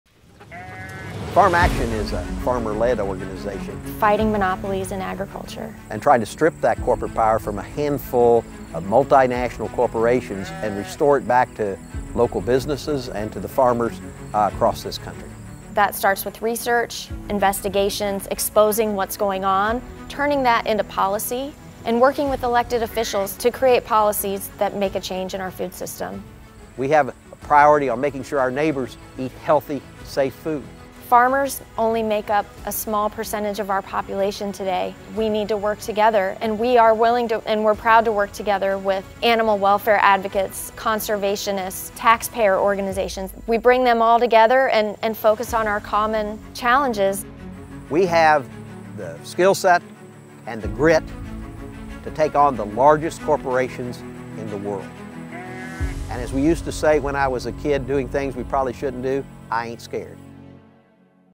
Why We Fight: Empowering Farmers
In this short clip, farmers and co-founders Angela Huffman and Joe Maxwell share Farm Action's vision for a fair and sustainable food system.